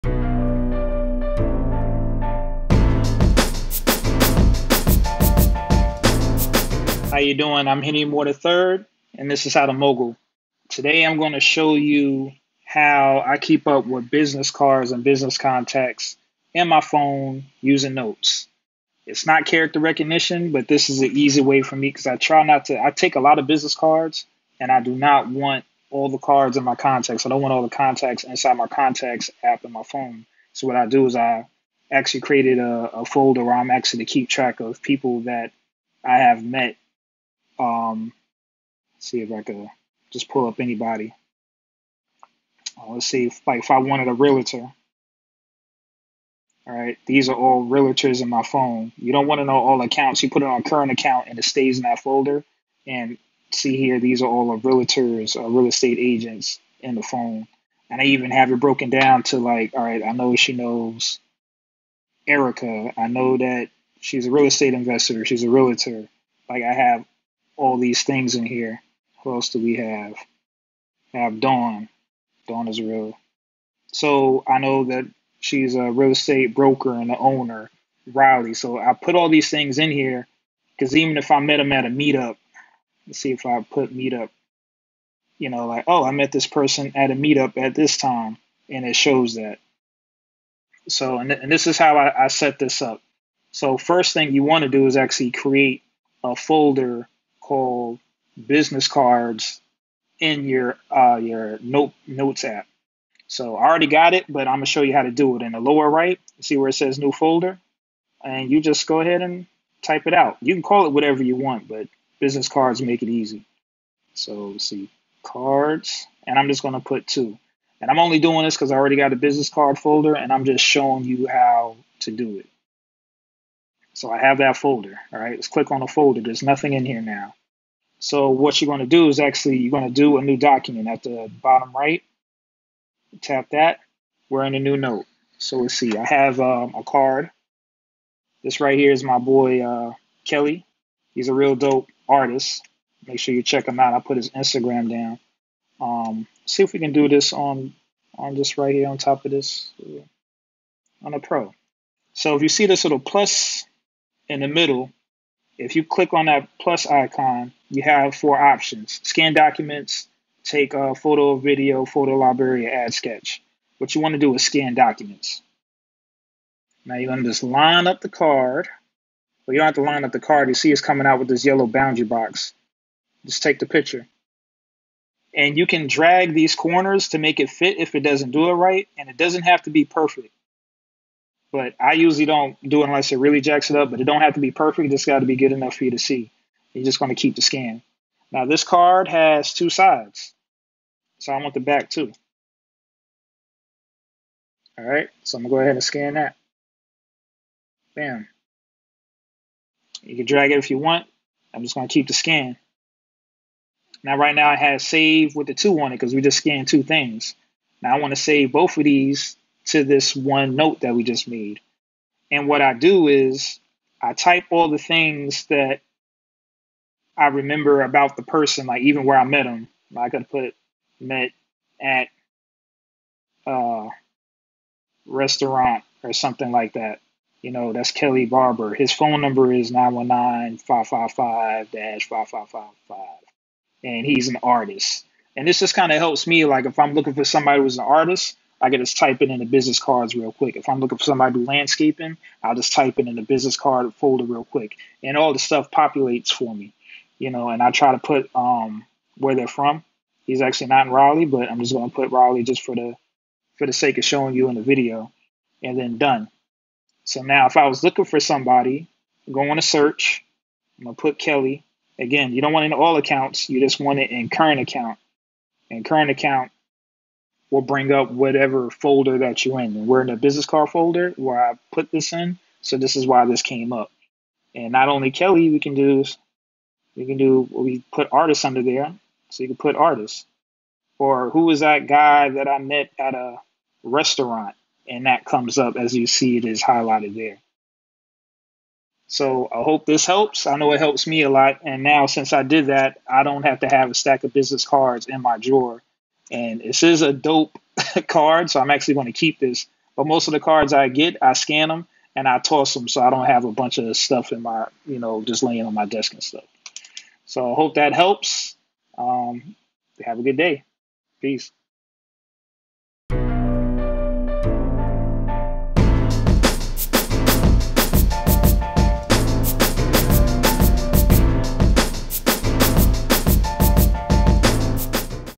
How to Add Business Cards to iPhone XS iOS 12
In this video I will show you How to Add Business Cards to iPhone XS iOS 12

I will show you how to scan a business card in with the notes app using an iPhone X but this will work if you're using iPhone XS, iPhone XS Max or any iPhone running iOS 11 or iOS 12.

This is a nice tip and trick using the Notes app on the iPhone I document scan the cards into the notes app so it's easily accessible on my iPhone, iPad or MacBook Pro very easy and I don't have to add a bunch of contacts in my iPhone.

This is not OCR but it’s an easy way to scan and add business cards to your iPhone. Sorry about the sound. I really couldn’t use my mic without it getting in the way of the recording.

It would be nice if Apple had a feature to scan business cards and it will add it on it’s own but we are not there yet.